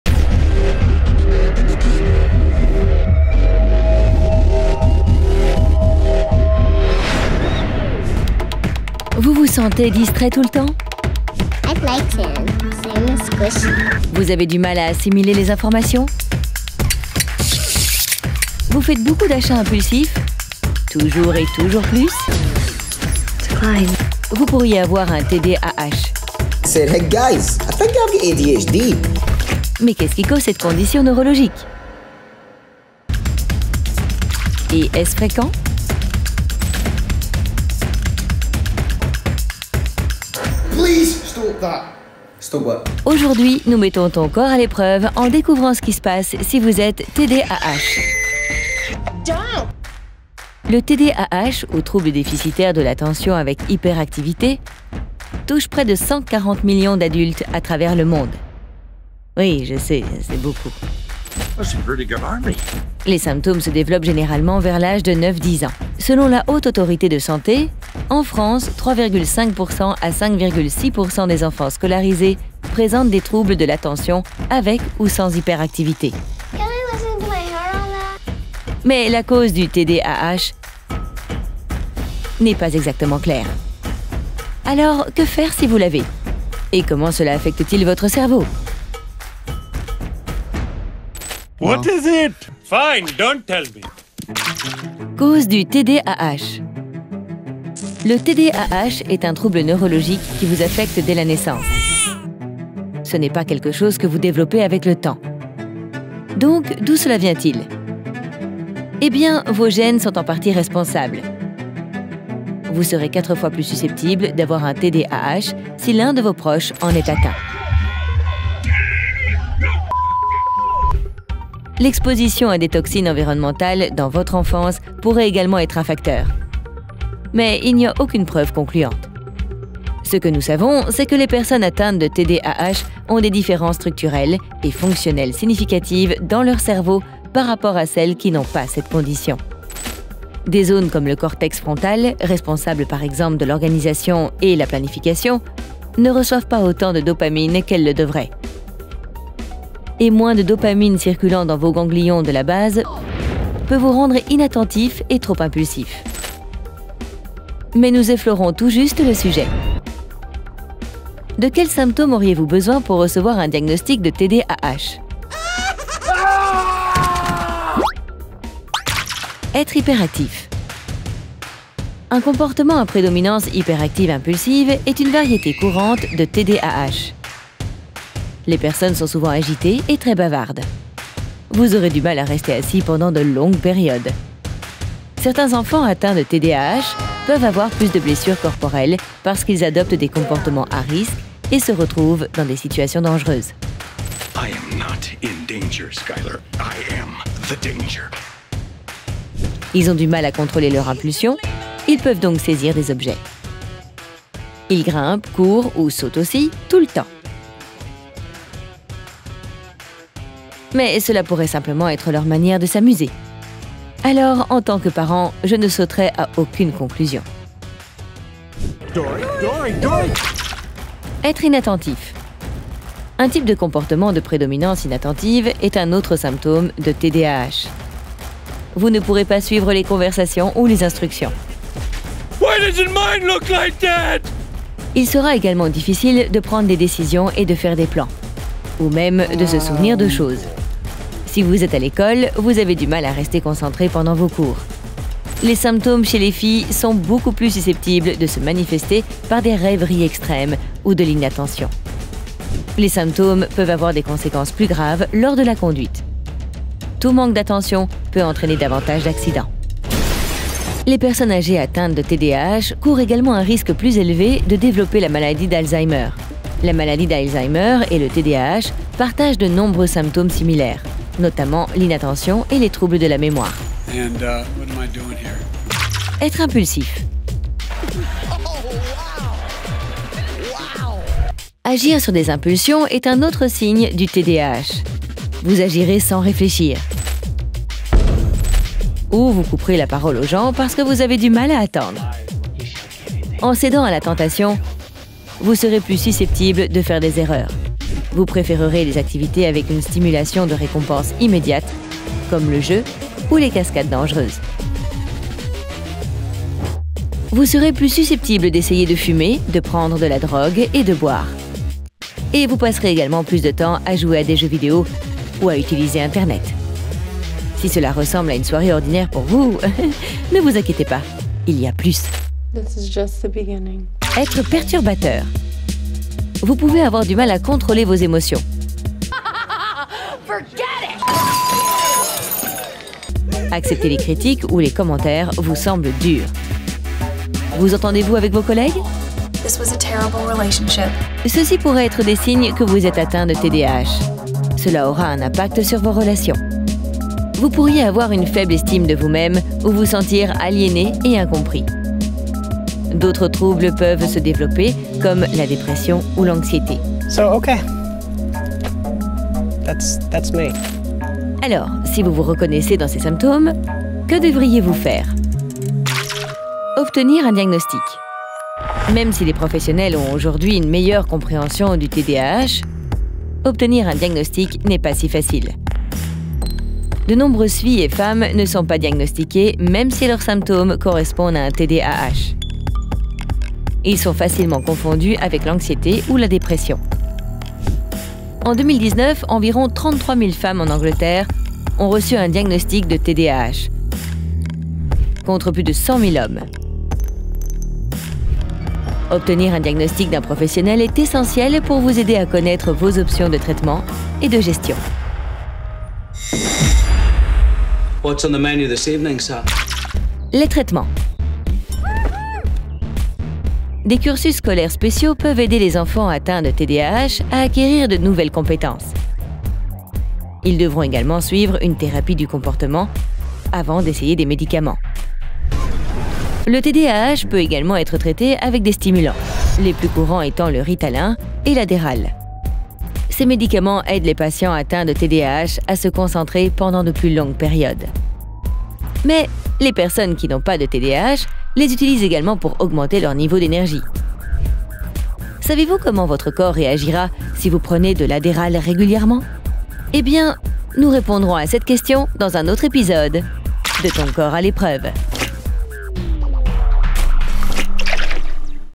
Avez-vous un TDAH ?
Vous vous sentez distrait tout le temps ? Vous avez du mal à assimiler les informations ? Vous faites beaucoup d'achats impulsifs ? Toujours plus ? Vous pourriez avoir un TDAH. Mais qu'est-ce qui cause cette condition neurologique ? Et est-ce fréquent ?